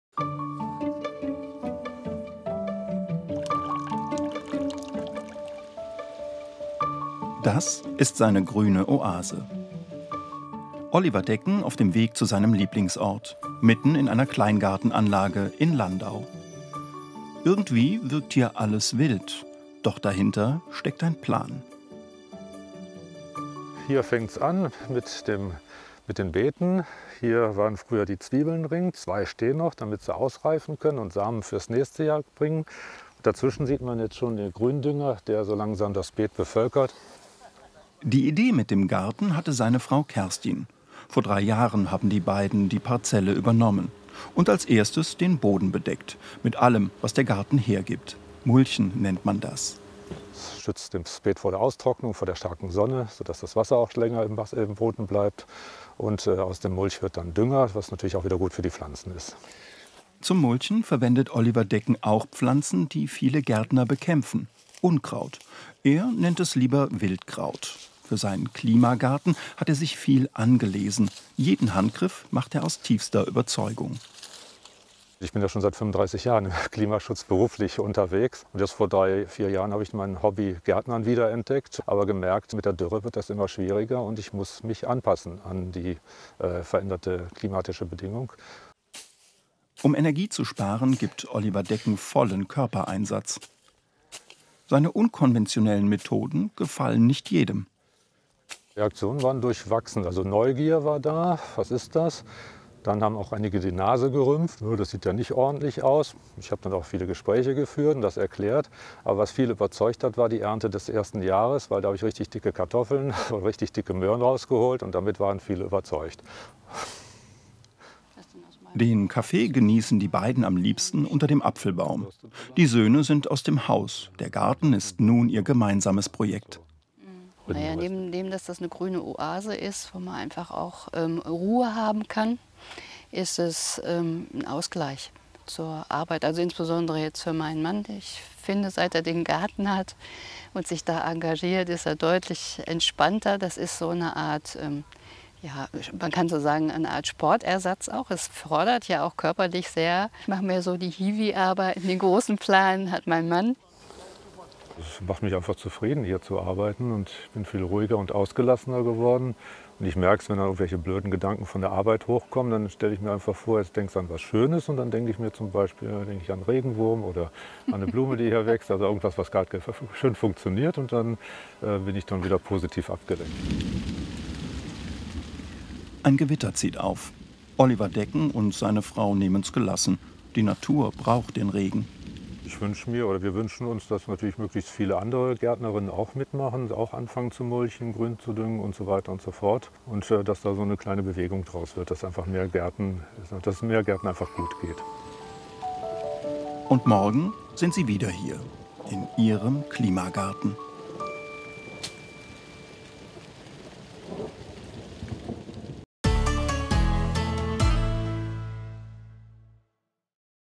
Gärtnern mit der Natur - der Klimagarten von Oliver in Landau
Wie gärtnert man eigentlich angepasst an die Natur und an den Klimawandel? Oliver hat dies in seinem Schrebergarten toll umgesetzt. Die Nachbarn waren zuerst nicht begeistert, aber die erste Ernte hat alle überzeugt. Ein toller Beitrag vom SWR. Quelle